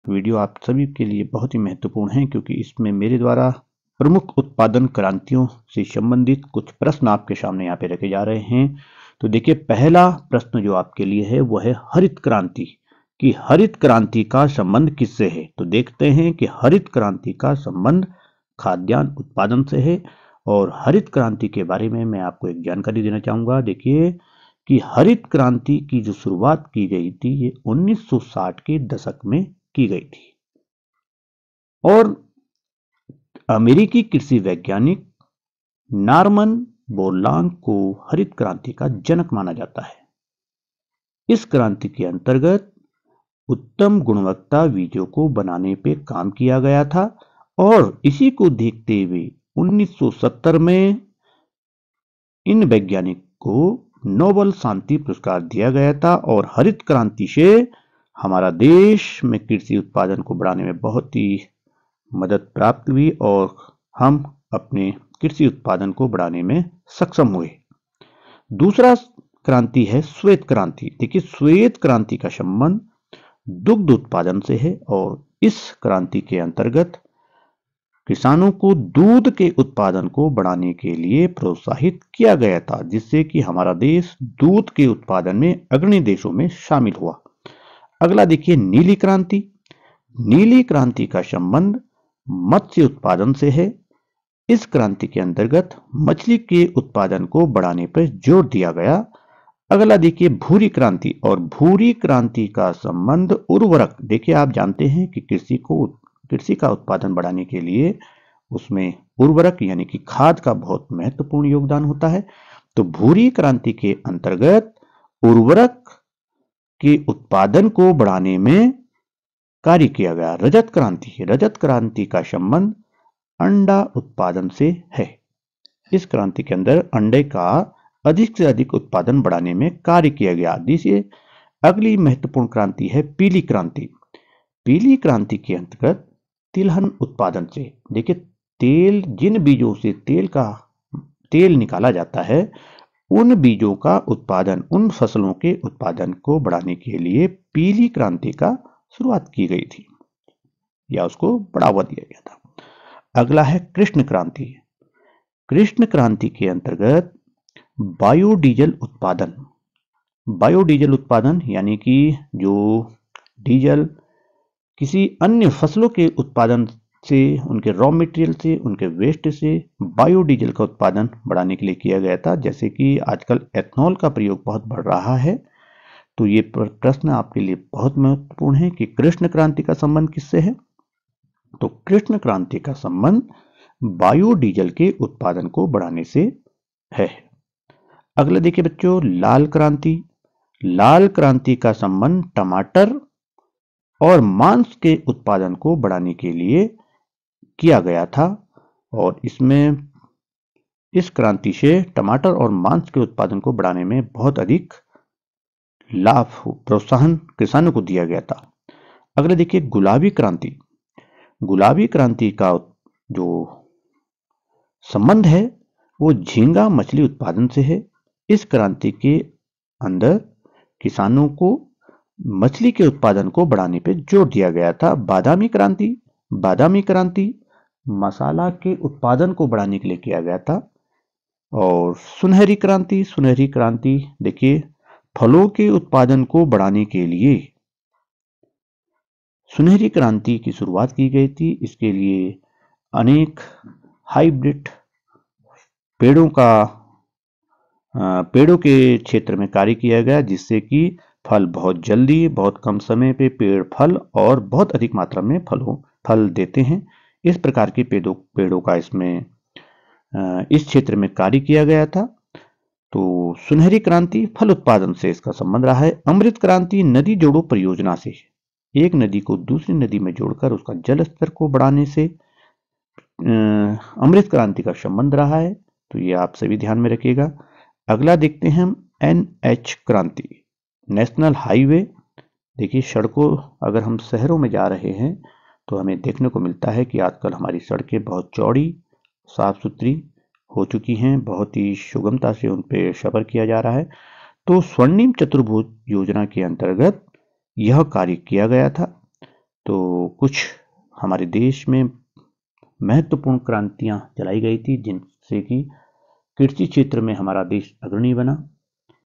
सामान्य ज्ञान/प्रमुख उत्पादक क्रांतियां/हरित क्रांति कृष्ण क्रांति गुलाबी क्रांति पीली क्रांति सफेद
इस वीडियो के अंतर्गत स्वतंत्र भारत में चलाई गई अनेक प्रमुख उत्पादक क्रांतियां से संबंध रखा गया है हमारे देश में विभिन्न विभिन्न समय में अनेक प्रकार की उत्पादन क्रांतियां चलाई गई जिससे कि हमारा देश कृषि उत्पादन के क्षेत्र पर आत्मनिर्भर हुआ है।